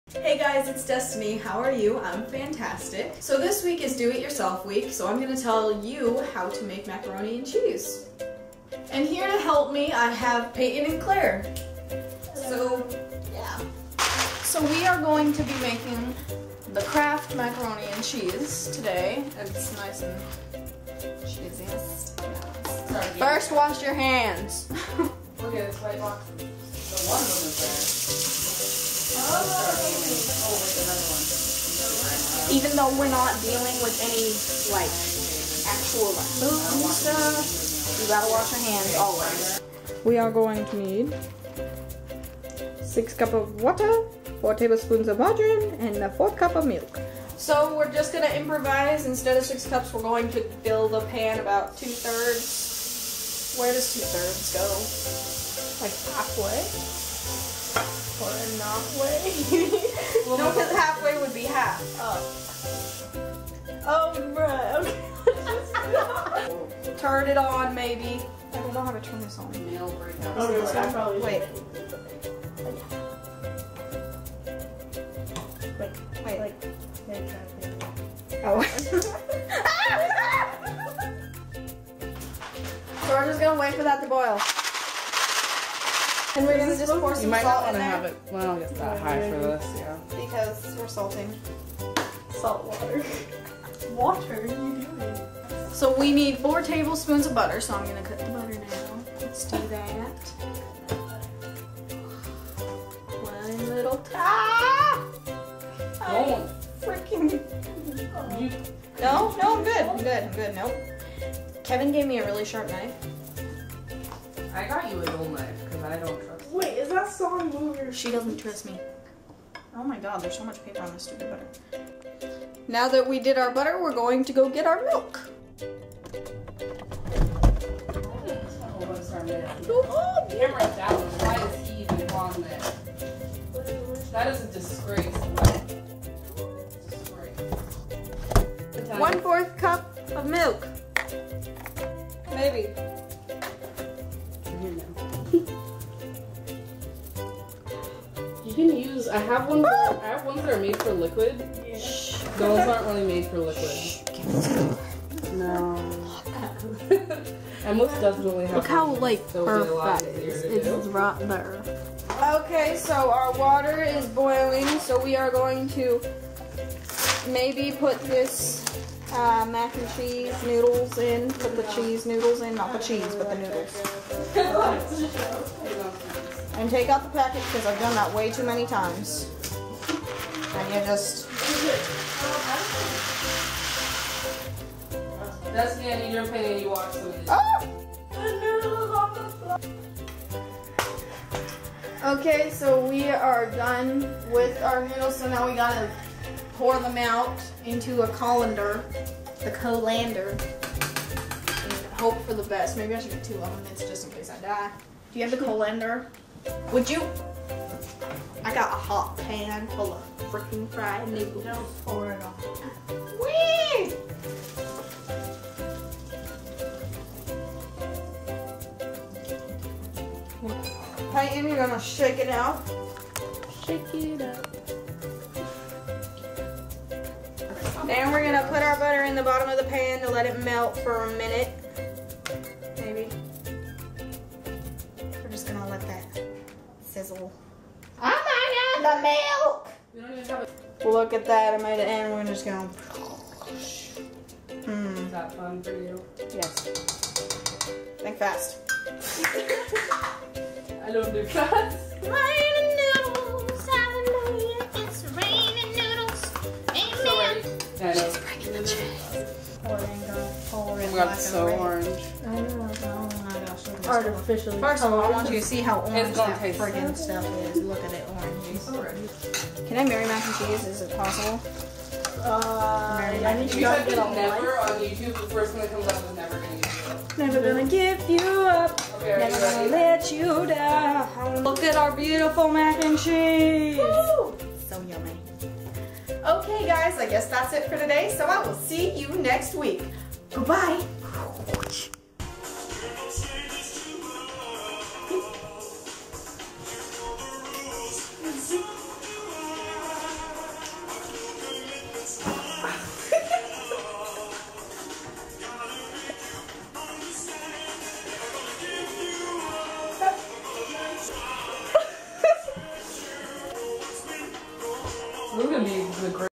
How To Make Mac And Cheese | Destiny Leslie
Hey guys!

So this week is DIY week, and I just thought I'd show you how to make some mac and cheese because who doesn't love a good mac and cheese? Hope you enjoy it!

I would love to talk to all of you. :)

Have a wonderful day, lovelies. :)